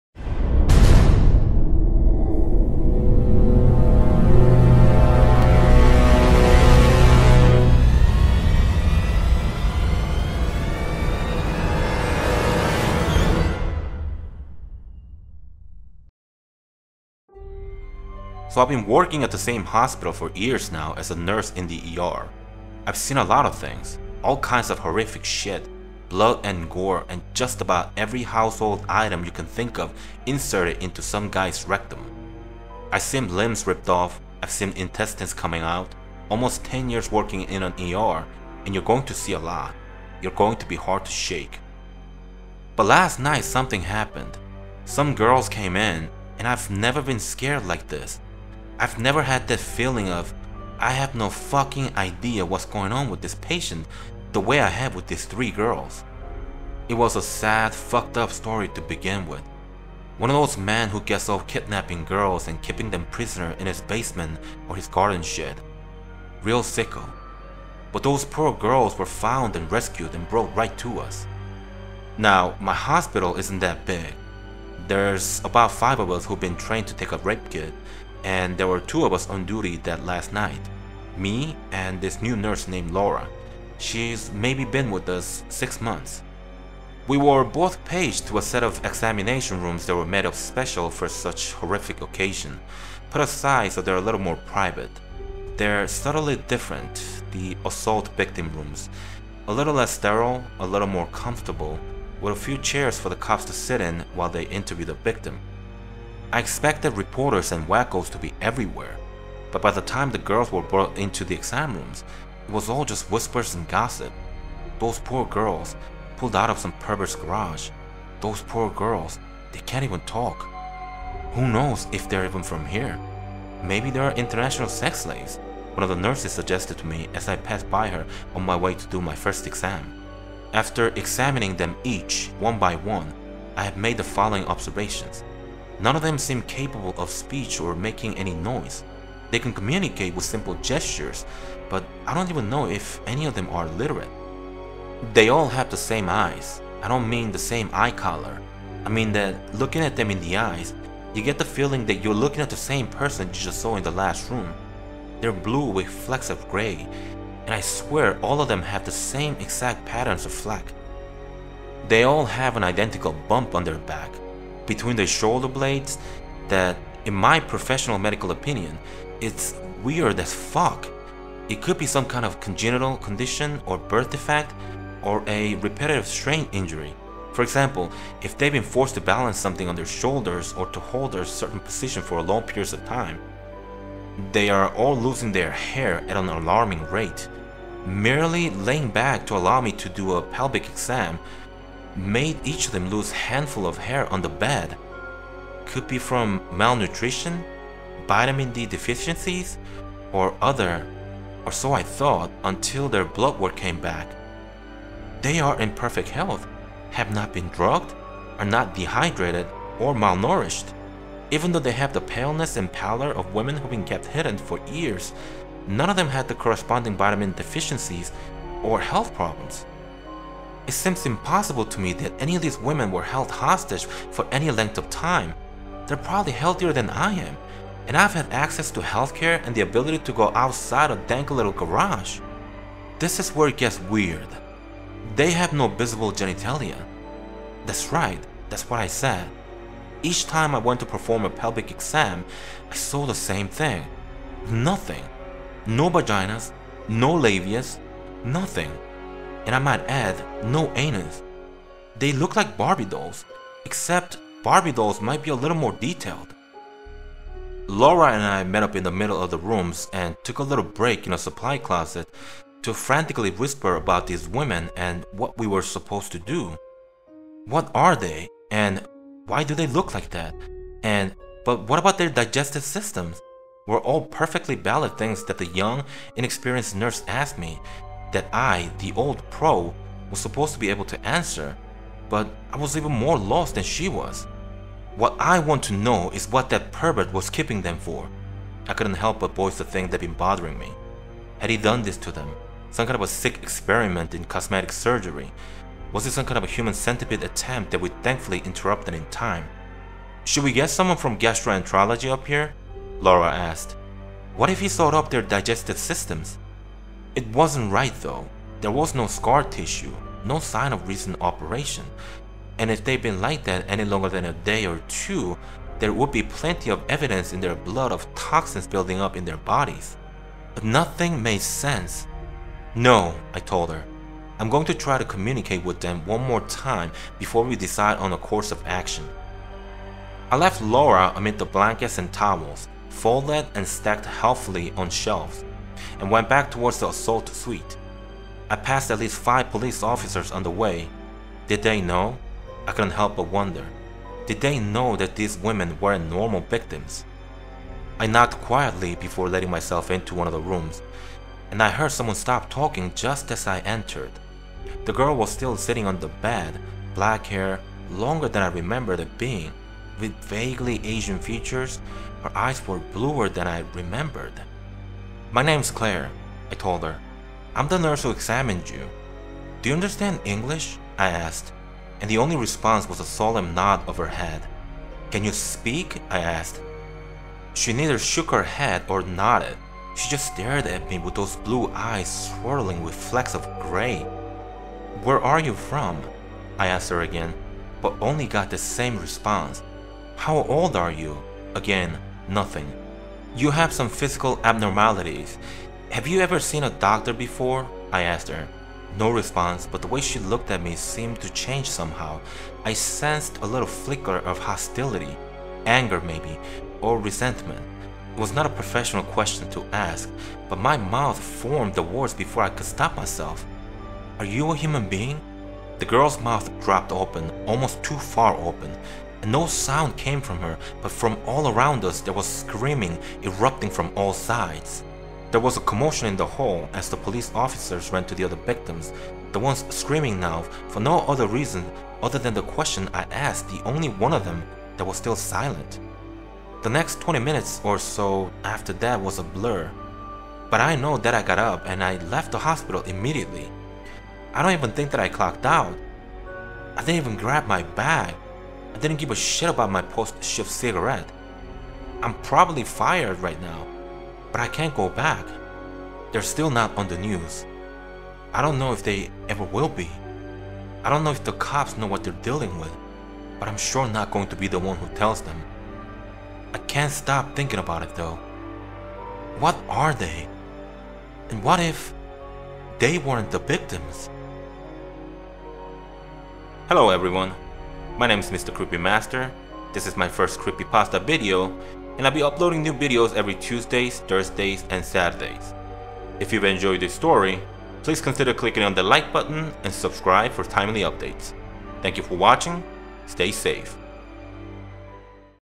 "The Three Girls In The ER" - True Scary Stories From Reddit
Three mysterious young girls, presumably victims of abuse by their kidnapper are brought into the ER. The ensuing happenings only deepen the mystery of the girls...